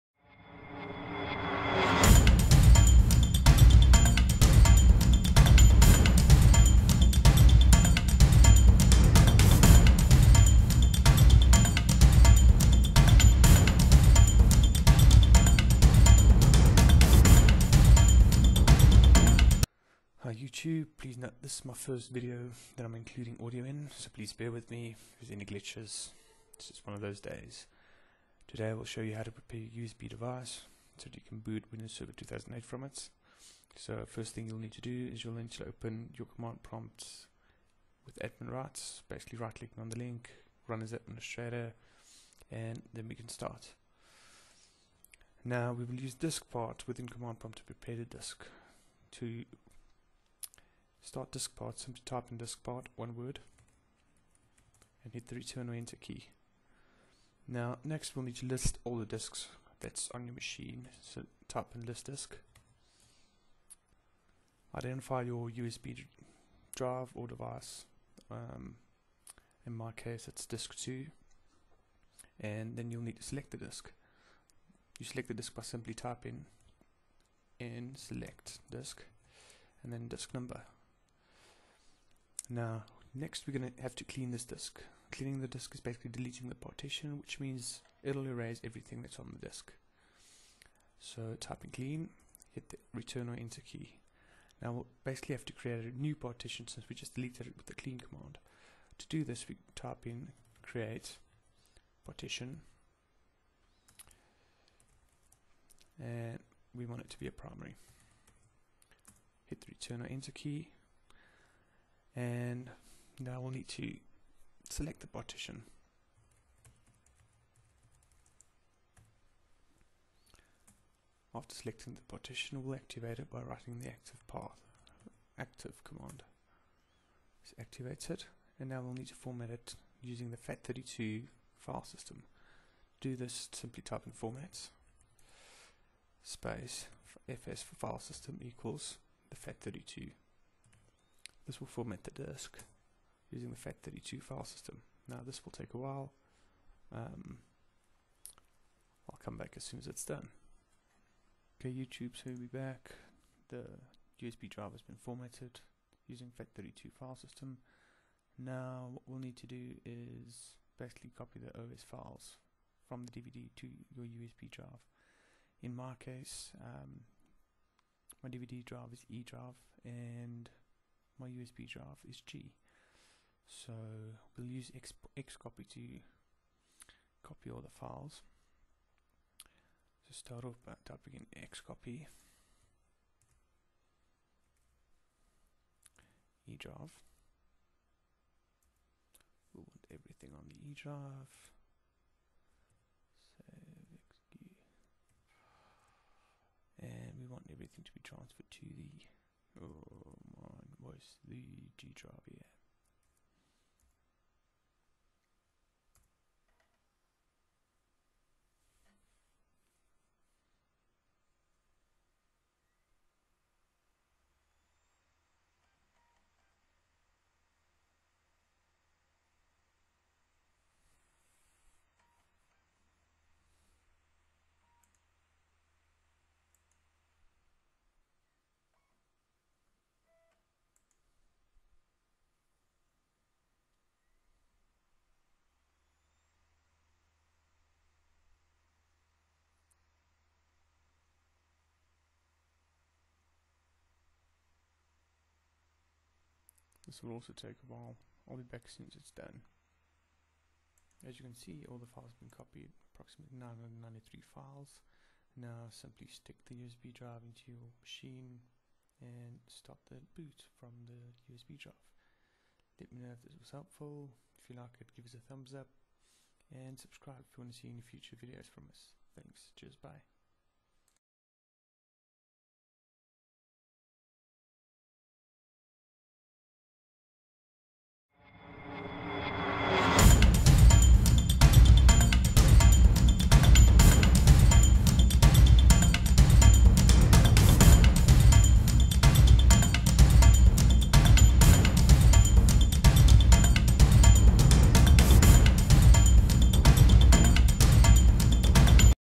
Installing Server 2008 from usb drive or stick
Basically what we will do is to prepare your usb disk so you can boot windows server 2008 from it